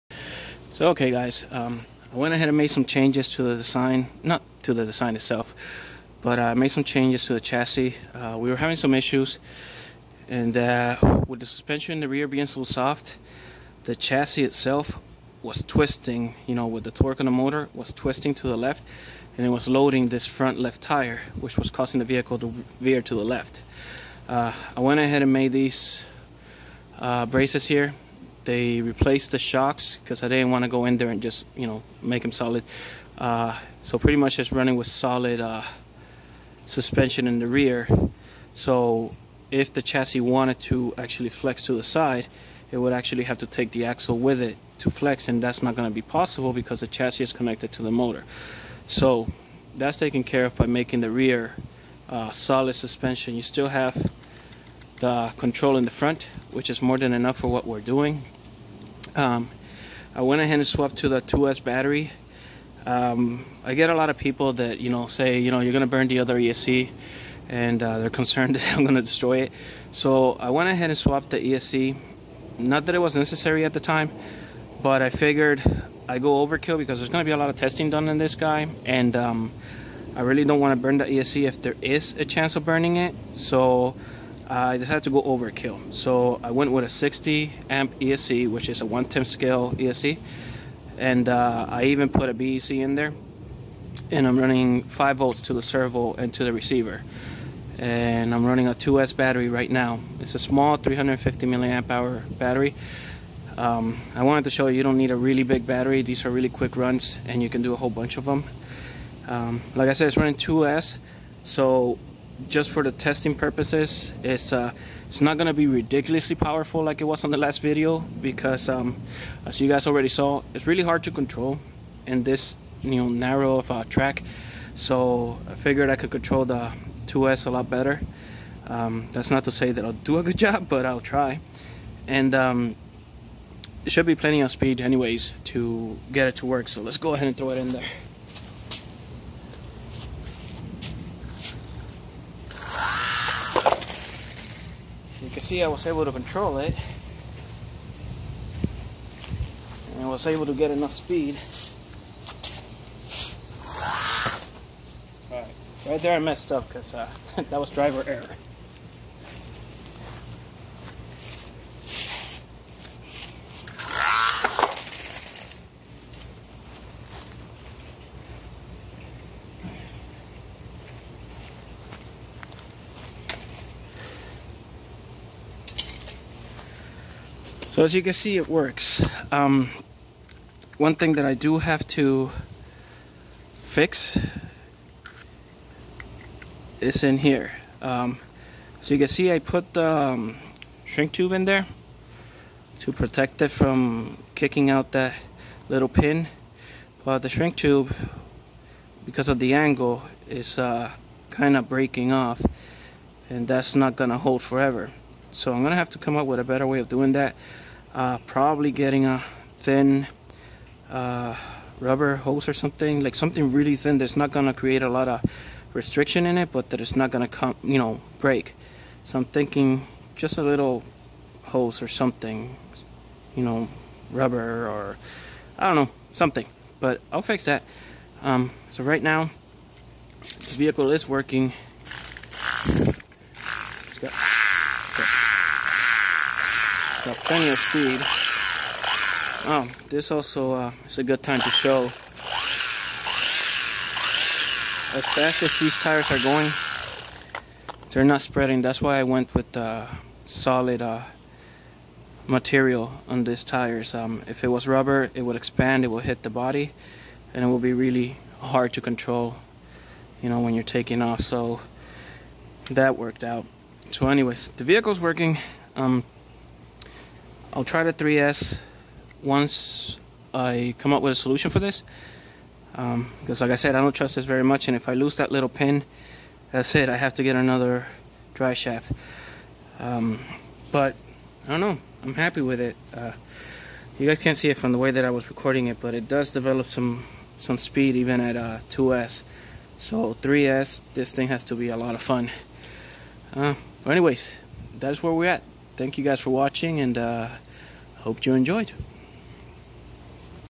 RC4WD 1/18 Gelande II mud racer sand test 2
Fixed the torque steer problem. Upgraded to 60a esc with external bec in order to go overkill just in case to protect all the electronics since there much to be tested still.